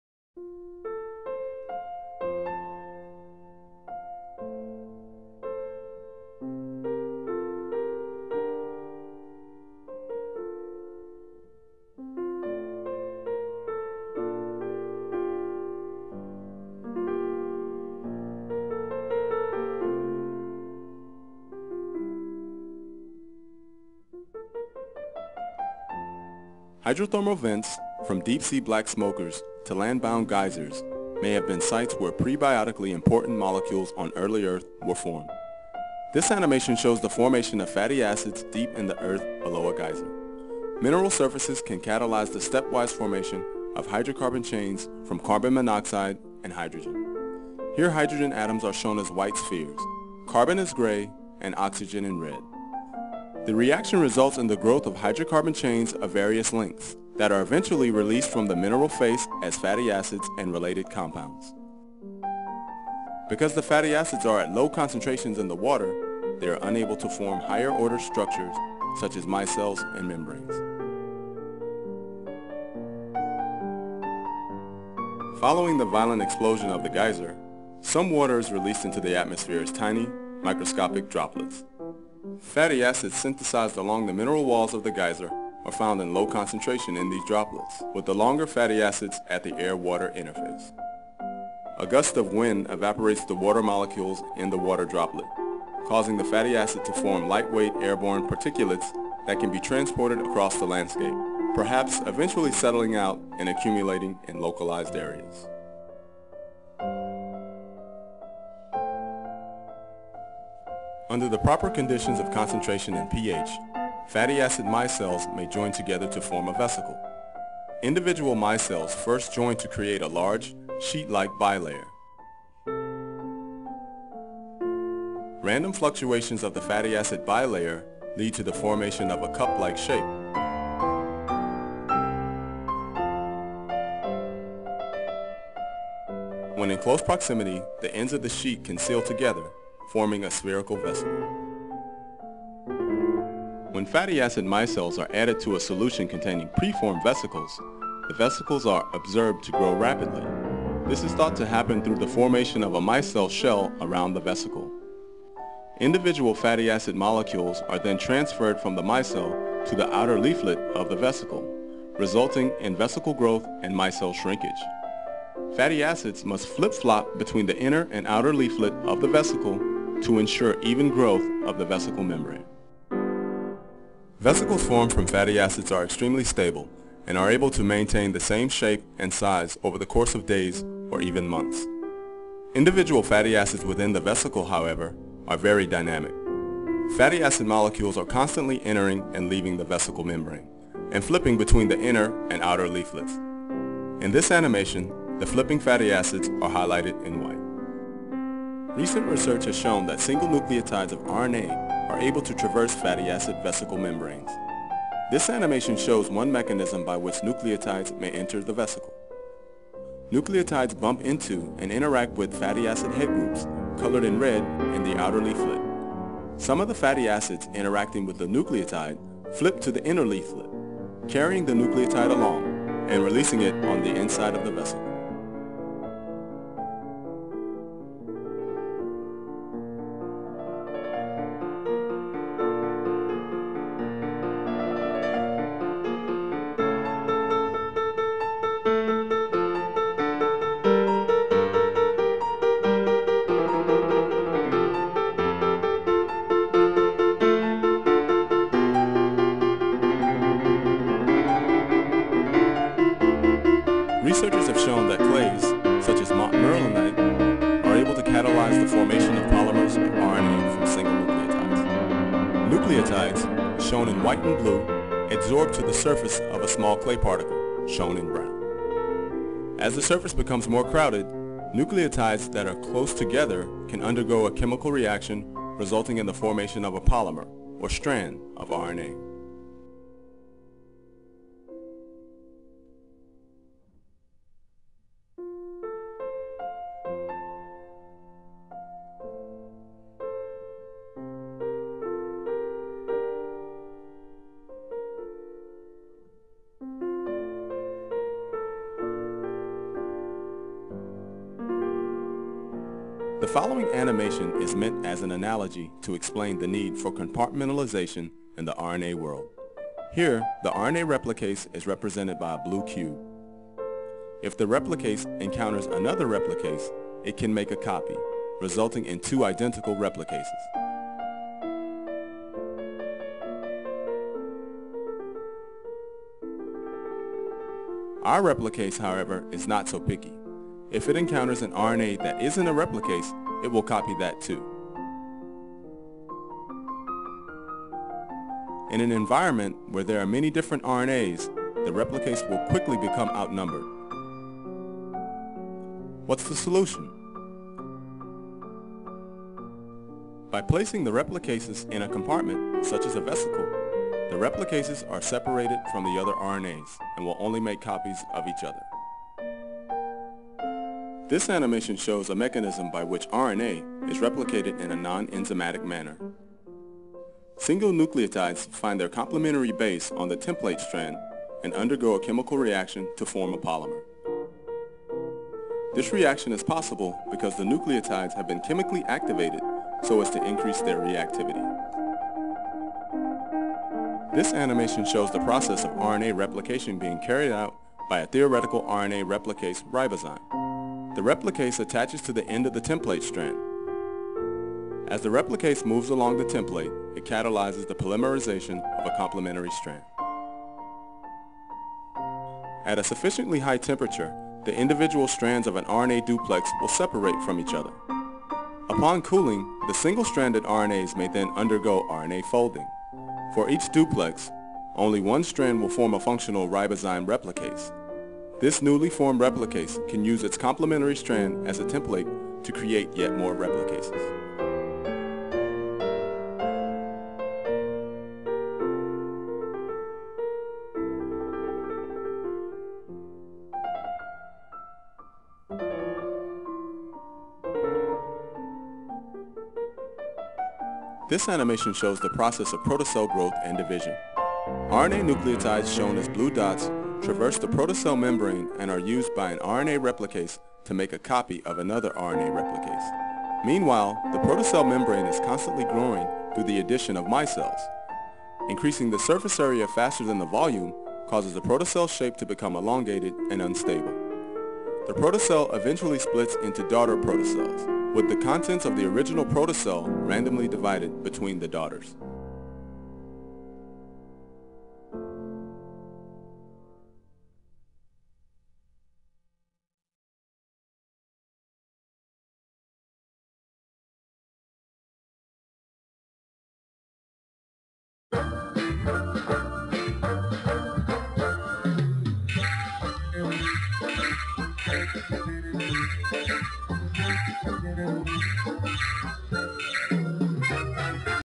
Abiogenesis - origin of life
All animations and are from the Exploring Life's Origins project

Please visit the Exploring Life's Origins website for more movies and information on life's origins and the RNA World.

Please note that animations in this video and on the Exploring Life's Origins website are licensed under a Creative Commons License, and may be freely downloaded for non-commercial uses with proper attribution.

Animations were made by Janet Iwasa, with narration by Tony Bell.

Included some objections made by creationists and the rebuttal to them.

looks familiar? Yep it is a different version of CDK007's classic video on the topic. I used his source original videos.